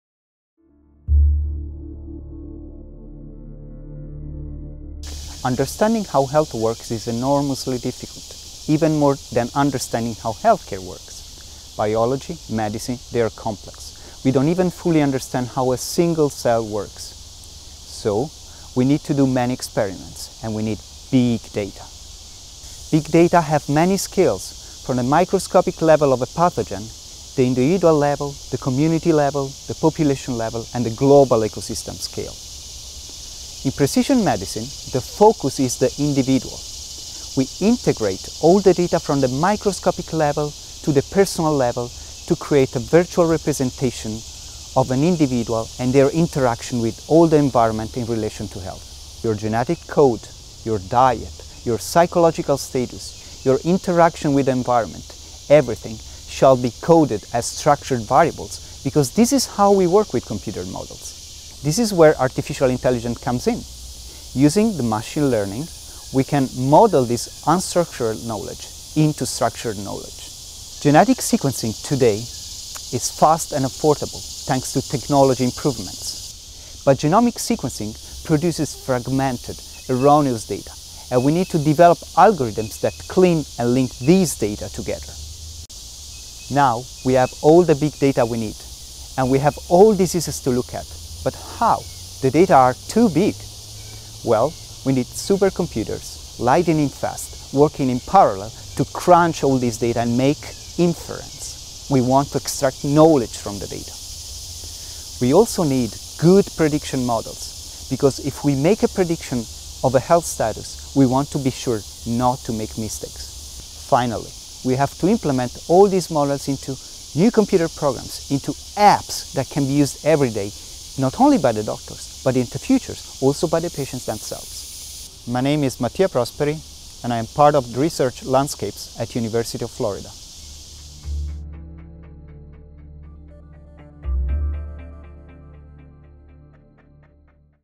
University of Florida Research Landscapes: Mattia Prosperi, M.Eng, Ph.D.
Dr. Mattia Prosperi, associate professor the in the University of Florida department of epidemiology, discusses his research in the areas of biomedical modelling and ‘big data’ mining focused on precision medicine and translational science.